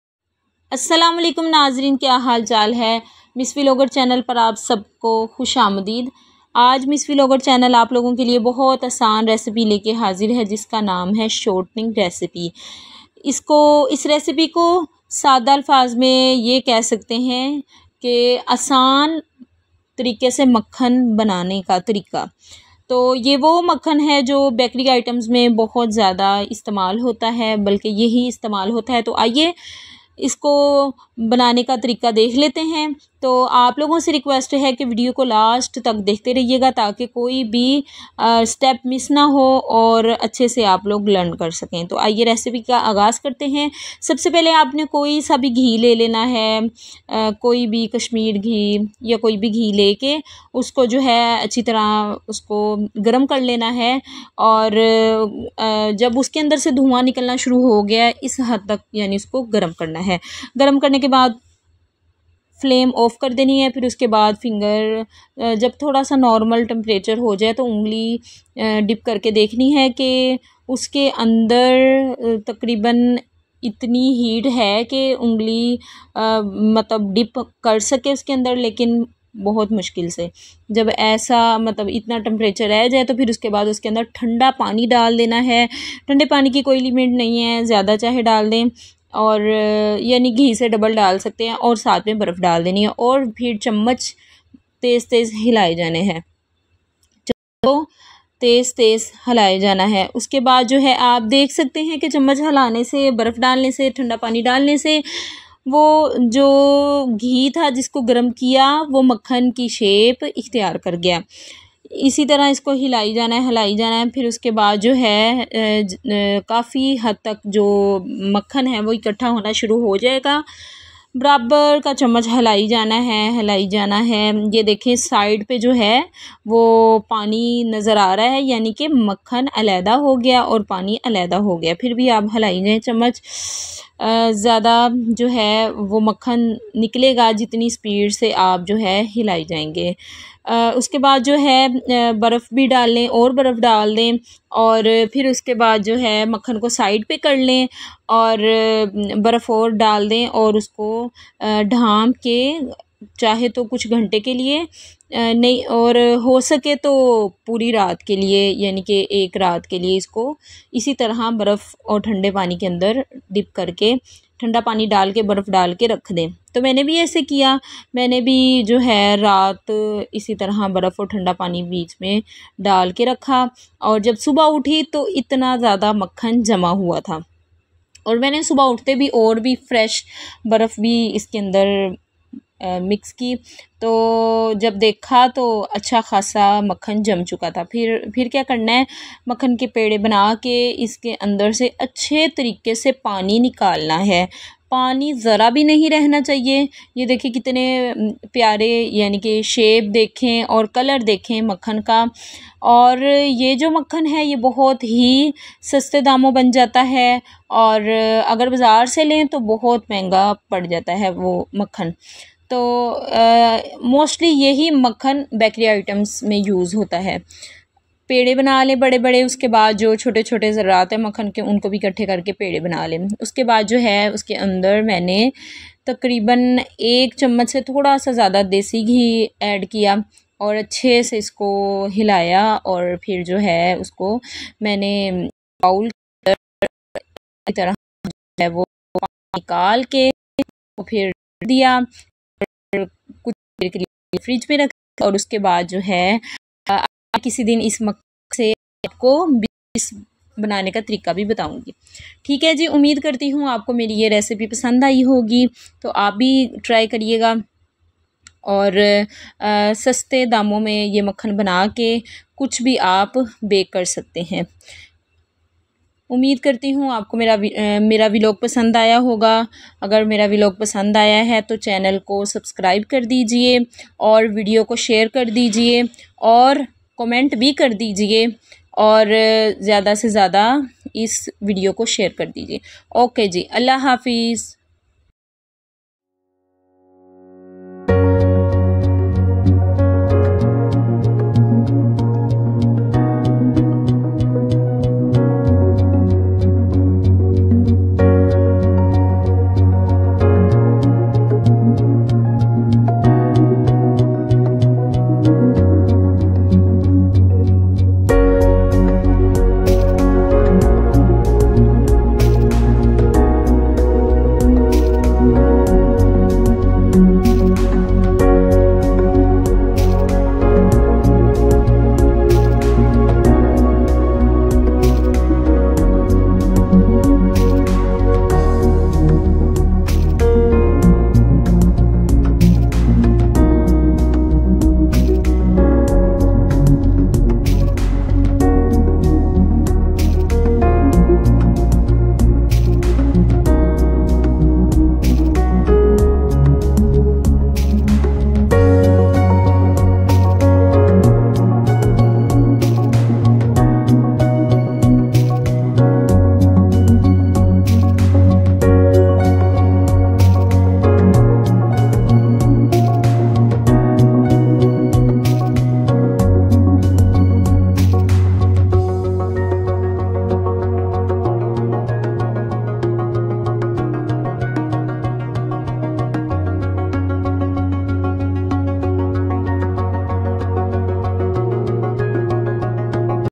Kashmir ghee Sy butter bnany ka asan triqa.||MT store||Miss Vlogger
I made shortening with Kashmir ghee ,ice,cool water and desi ghee.I will bake through shortening biscuits.We can easily made shortening at home in some money.